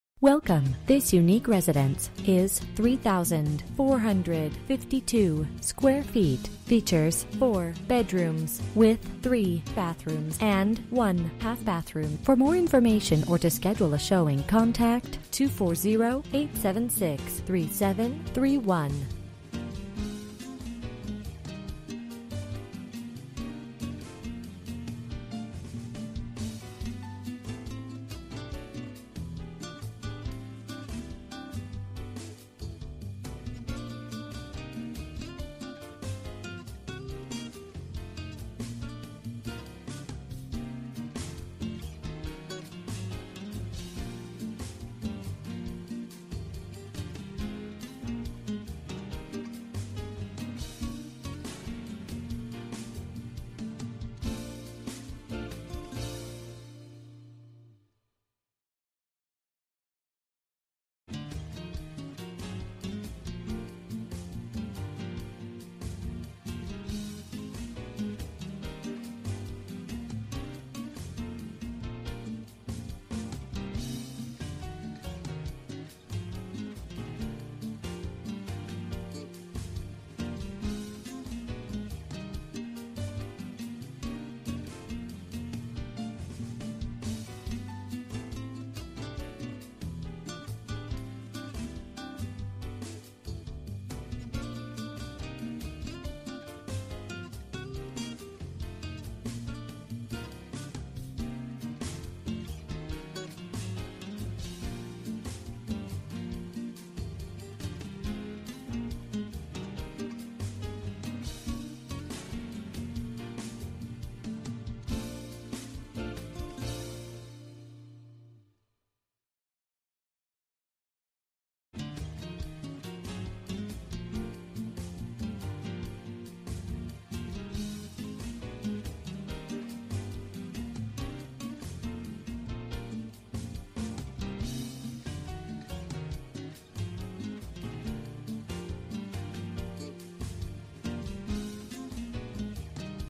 Residential for sale - 15704 Henrietta Drive, Accokeek, MD 20607
What a steal in one of  Accokeeks  most desired communities.Beautiful 4 bedroom home with over 3,776 sq ft of living space. Home boast large dining room, eat in kitchen, two large master bedrooms with walk in closets, Beautiful hot tub on extended deck that seats 8.Extended yard space for basketball court and many yard games. Beautiful sunroom. Basement with bdrm den full bath, kitchenette, and bar. Bonus rm  for theater room and bar.

Bedrooms: 4
Bathrooms: 3 full and 1 half
Square Feet: 3452
Price: $405,000

MLS ID: MDPG527830